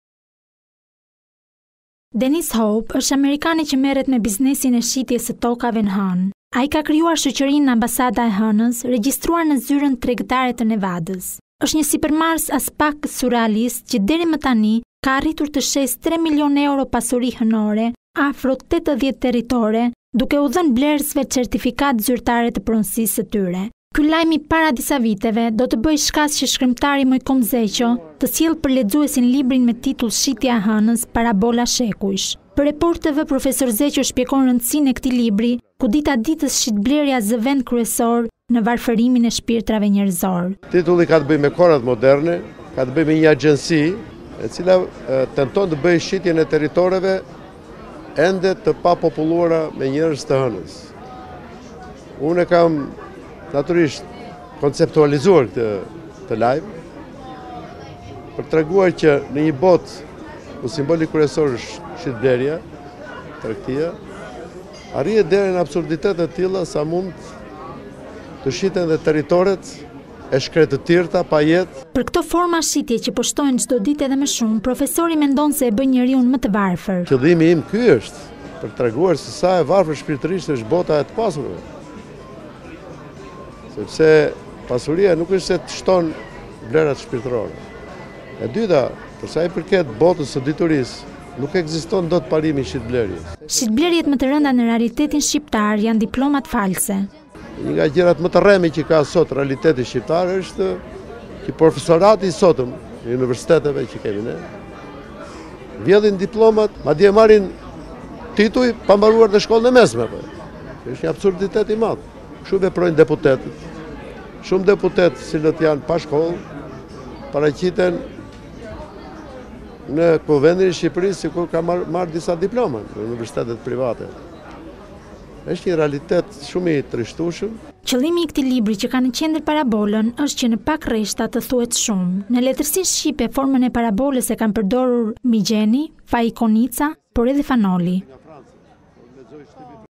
Report Tv - Shkrimtari Moikom Zeqo sjell për lexuesin librin “Shitja e Hënës” parabola shekujsh
Dennis Hope është amerikani që merret me biznesin e shitjes së tokave në Hënë. Ai ka krijuar shoqërinë “Ambasada e Hënës” regjistruar në zyrën tregtare të Nevadës.

Është një sipërmarrës aspak surrealist, që deri më tani ka arritur të shesë 3 mln euro pasuri hënore, (afro 80 territore), duke u dhënë blerësve certifikatë zyrtare të pronësisë së tyre.

Ky lajm i paradisaviteve do të bëhej shkas që shkrimtari Moikom Zeqo të sjellë për lexuesin librin me titull “Shitja e Hënës”, parabola shekujsh.

Për “Report Tv” profesor Zeqo shpjegon rëndësinë e këtij libri, ku dita-ditës shitblerja zë vend kryesor, në varfërimin e shpirtrave njerëzor

“Ky është një libër me tregime dhe parabola. Kam synuar një formë të shkurtër të narratives që është më simbolike, më shumë kuptimore edhe më semantike, siç është parabola. Tematikat në këtë libër janë të ndryshme, që nga ato antike, ka mesjetën dhe kohërat moderne. Titulli ka të bëjë me kohërat moderne. Me një agjenci, e cila nuk është shpikja ime, por është përtej imagjinatës, e cila është konfirmuar në botë. U jam referuar lajmeve të shtypit. Një agjenci, e cila tenton që të bëjë shitjen e territoreve ende të papopulluara me njerëz të hënës. Unë e kam konceptualizuar këtë lajm, për të treguar se në një botë ku simboli kryesor është shitblerja, tregtia arrihet deri në absurditete të tilla, sa mund të shiten edhe territoret e shkretëtirëta pa jetë, pa  ajër të hënës. Në një parabolë të tillë, inextremis është gati-gati në kufi të absurdit ku mund të kuptohet diçka nga realiteti i botës së sotme. Njerëzit revoltohen ndaj këtij realiteti, ku çdo gjë që konsiderohet shitblerje, ku të gjitha marrëdhëniet njerëzore shndërrohen në marrëdhënie monetare”.